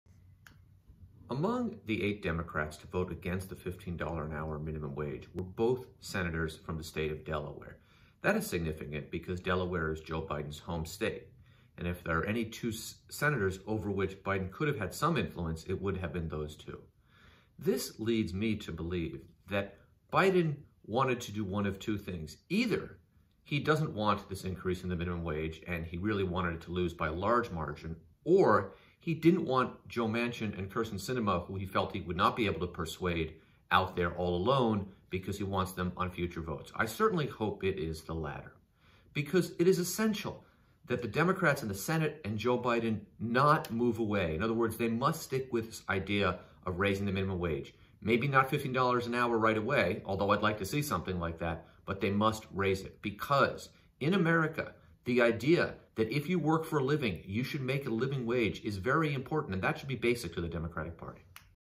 Mitchell Minute 587-Both Delaware Senators Voted Against the Minimum Wage Increase
March 6, 2021-It is significant that both Democratic senators from Delaware voted against the $15/hour minimum wage. There are several possible reasons why they might have voted against the increase, but if Joe Biden has influence over any senators, it is those two.